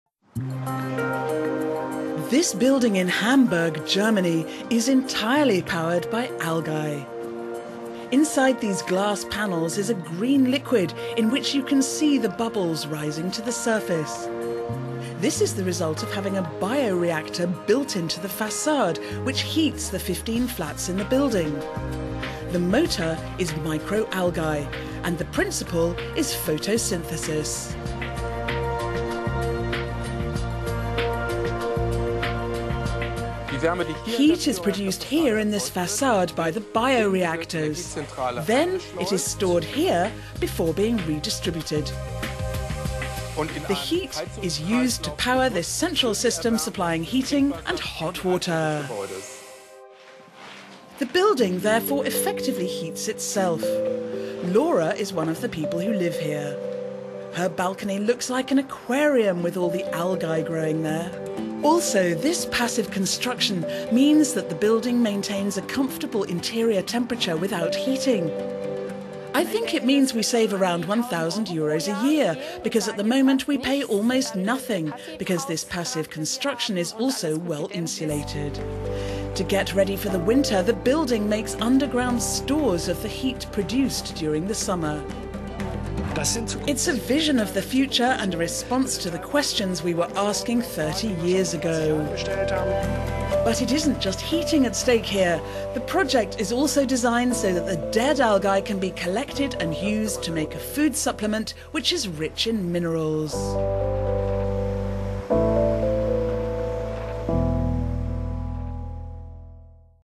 It's Alive! The world's first algae-powered building
There is a building in Hamburg, Germany, which is entirely powered by algae. Inside the glass panels is a green liquid in which you can see little bubbles rising to the surface. This is the result of having a bio-reactor built into the facade, which heats the 15 flats in the building. The motor is micro algae, and the principle is photosynthesis.